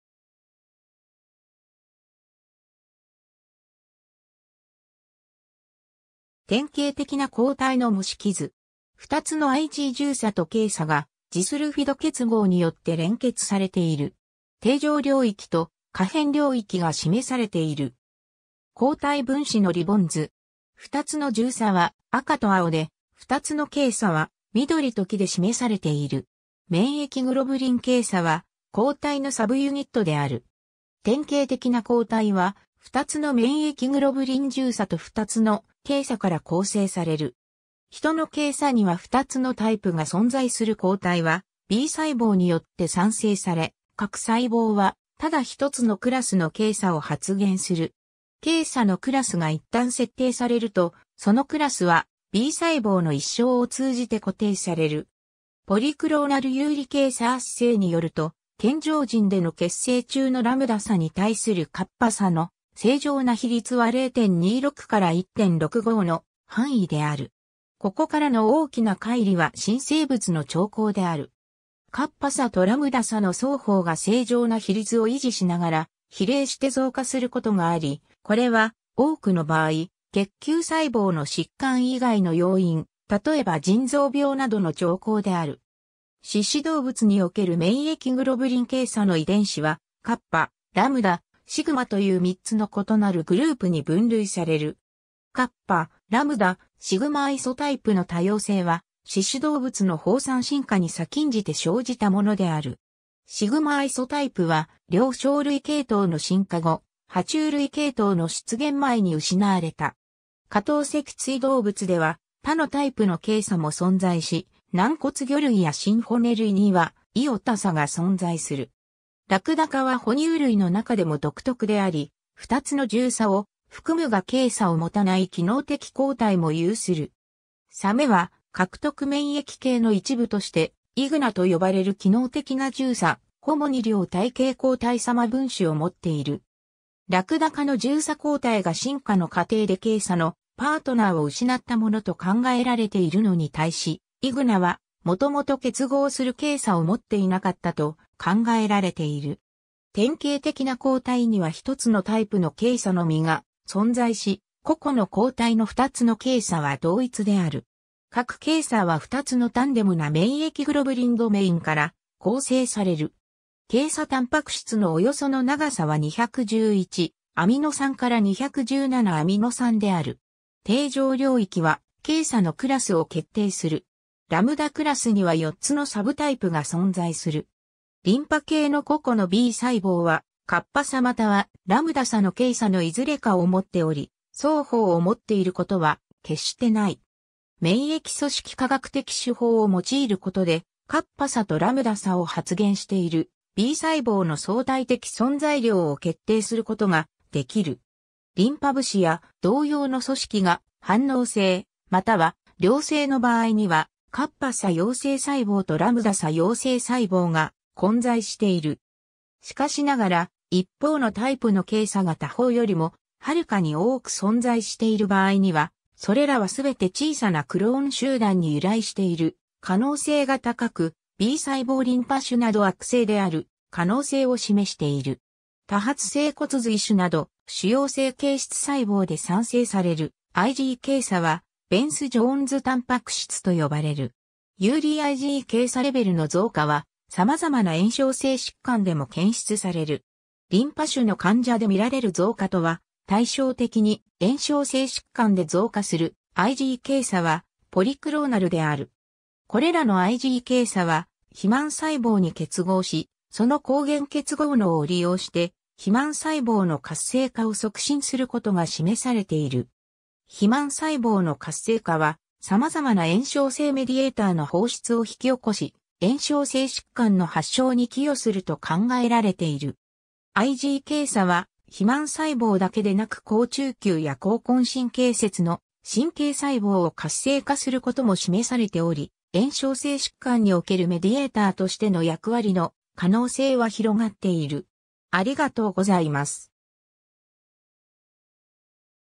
免疫グロブリン軽鎖
典型的な抗体の模式図。
2つのIg重鎖（青）と軽鎖（緑）がジスルフィド結合によって連結されている。
定常領域（C）と可変領域（V）が示されている。
 抗体分子のリボン図。
2つの重鎖は赤と青で、2つの軽鎖は緑と黄で示されている。
 免疫グロブリン軽鎖（めんえきグロブリンけいさ、英: immunoglobulin light chain）は、抗体（免疫グロブリン）のサブユニットである。
典型的な抗体は、2つの免疫グロブリン（Ig）重鎖と2つの軽鎖から構成される。
 ヒトの軽鎖には2つのタイプが存在する 抗体はB細胞によって産生され、各細胞はただ1つのクラスの軽鎖を発現する。
軽鎖のクラスがいったん設定されると、そのクラスはB細胞の一生を通じて固定される。
ポリクローナル遊離軽鎖アッセイによると、健常人での血清中のλ鎖に対するκ鎖の正常な比率は0.26から1.65の範囲である。
ここからの大きな乖離は新生物の徴候である。
κ鎖とλ鎖の双方が正常な比率を維持しながら比例して増加することがあり、これは多くの場合、血球細胞の疾患以外の要因、たとえば腎臓病などの徴候である。
 四肢動物における免疫グロブリン軽鎖の遺伝子は、κ、λ、σという3つの異なるグループに分類される。
κ、λ、σアイソタイプの多様性は四肢動物の放散進化に先んじて生じたものである。
σアイソタイプは両生類系統の進化後、爬虫類系統の出現前に失われた。
 下等脊椎動物では他のタイプの軽鎖も存在し、軟骨魚類や真骨類にはι鎖が存在する。
 ラクダ科は哺乳類のなかでも独特であり、2つの重鎖を含むが軽鎖を持たない機能的抗体も有する。
 サメは獲得免疫系の一部として、IgNAR（immunoglobulin new antigen receptor)）と呼ばれる機能的な重鎖ホモ二量体型抗体様分子を持っている。
ラクダ科の重鎖抗体が進化の過程で軽鎖のパートナーを失ったものと考えられているのに対し、IgNARはもともと結合する軽鎖を持っていなかったと考えられている。
 典型的な抗体には1つのタイプの軽鎖のみが存在し、個々の抗体の2つの軽鎖は同一である。
 各軽鎖は2つのタンデムな免疫グロブリンドメインから構成される。
 軽鎖タンパク質のおよその長さは211アミノ酸から217アミノ酸である。
定常領域は軽鎖のクラス（κ、λ）を決定する。
λクラスには4つのサブタイプ（λ1、λ2、λ3、λ7）が存在する。
 リンパ系の個々のB細胞はκ鎖またはλ鎖の軽鎖のいずれかを持っており、双方を持っていることは決してない。
免疫組織化学的手法を用いることで、κ鎖とλ鎖を発現しているB細胞の相対的存在量を決定することができる。
リンパ節や同様の組織が反応性または良性の場合には、κ鎖陽性細胞とλ鎖陽性細胞が混在している。
しかしながら、一方のタイプの軽鎖が他方よりもはるかに多く存在している場合には、それらはすべて小さなクローン集団に由来している可能性が高く、B細胞リンパ腫など悪性である可能性を示している。
 多発性骨髄腫など、腫瘍性形質細胞で産生されるIg軽鎖はベンス・ジョーンズタンパク質と呼ばれる。
 遊離Ig軽鎖レベルの増加はさまざまな炎症性疾患でも検出される。
リンパ腫の患者でみられる増加とは対照的に、炎症性疾患で増加するIg軽鎖はポリクローナルである。
これらのIg軽鎖は肥満細胞に結合し、その抗原結合能を利用して肥満細胞の活性化を促進することが示されている。
肥満細胞の活性化はさまざまな炎症性メディエーターの放出を引き起こし、炎症性疾患の発症に寄与すると考えられている。
Ig軽鎖は肥満細胞だけでなく好中球や後根神経節の神経細胞を活性化することも示されており、炎症性疾患におけるメディエーターとしての役割の可能性は広がっている。